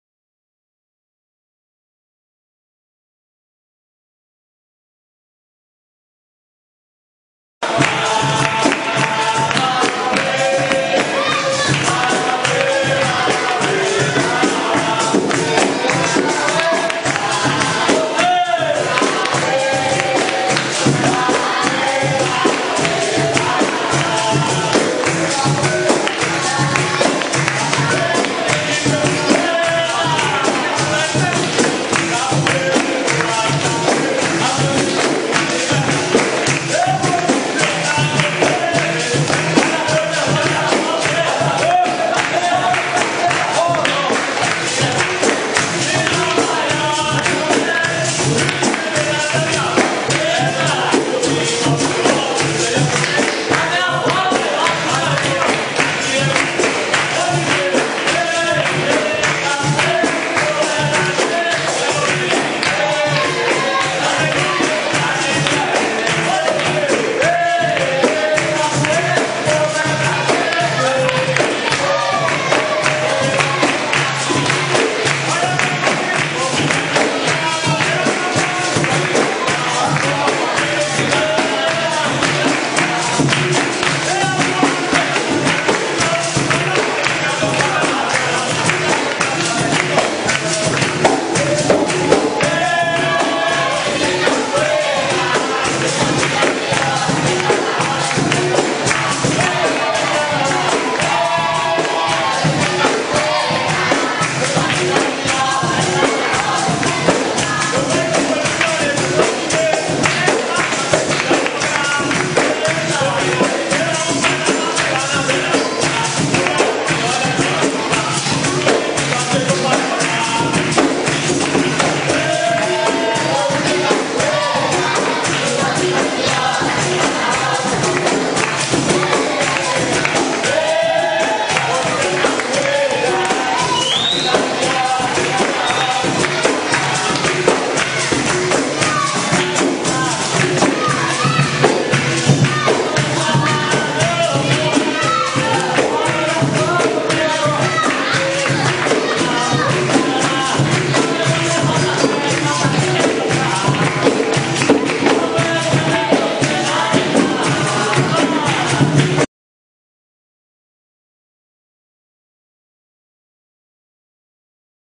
Capoeira Cobrangô
Grupo de capoeira que se reúne terças e quinta de 18h às 19h30 na Casa Brasil do Vila União, Fortaleza/CE.